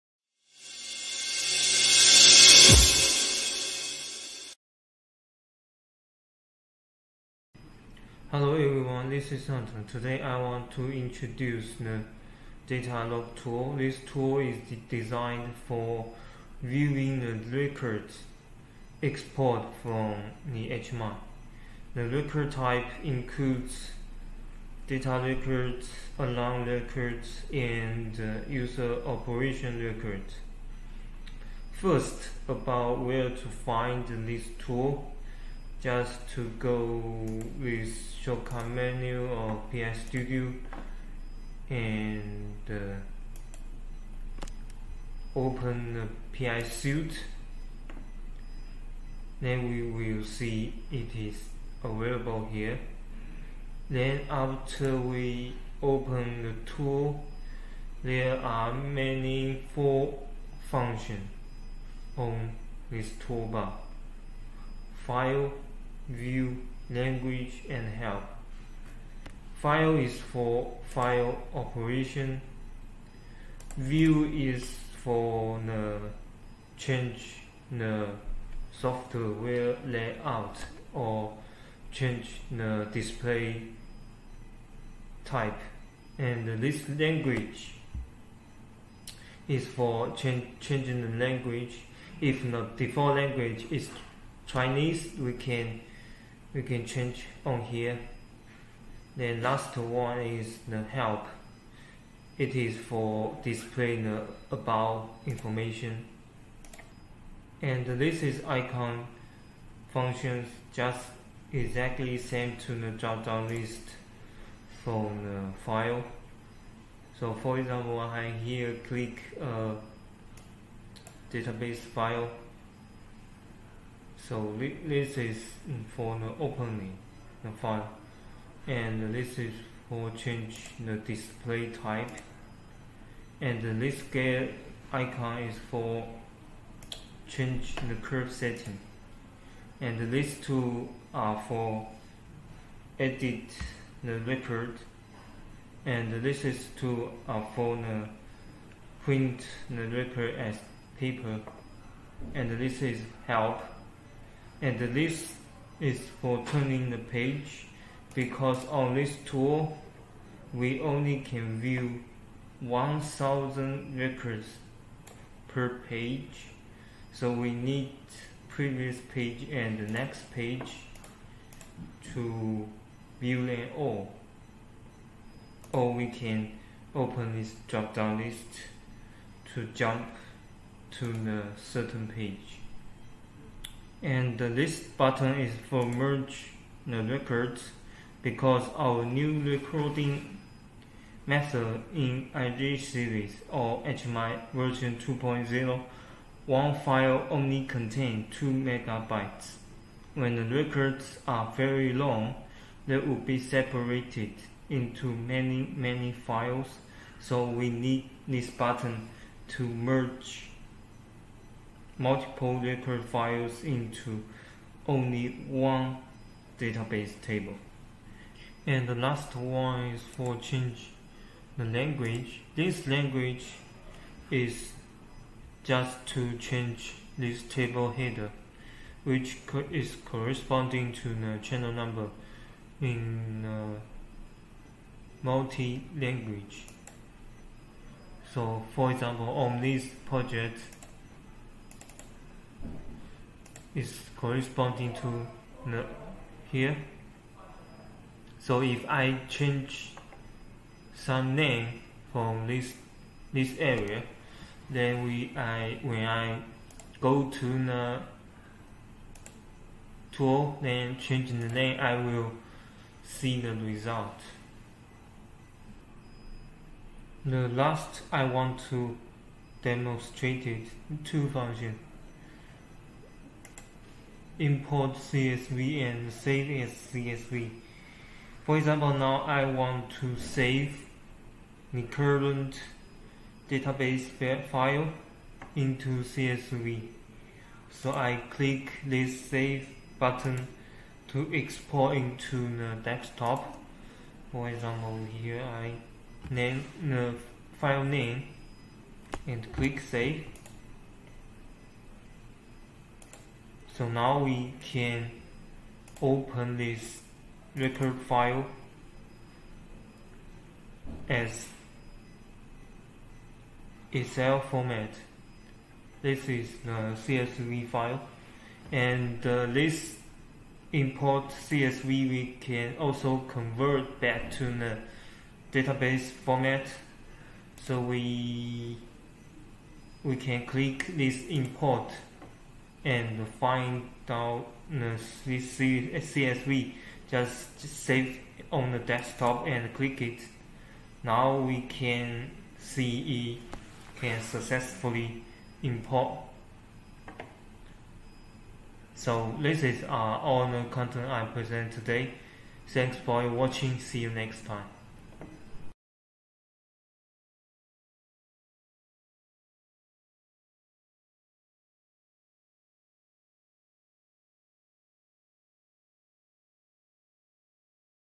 Wecon HMI Basic Object & Settings: 26. Data Log Tool
This tool is mainly designed for viewing the records exported from the HMI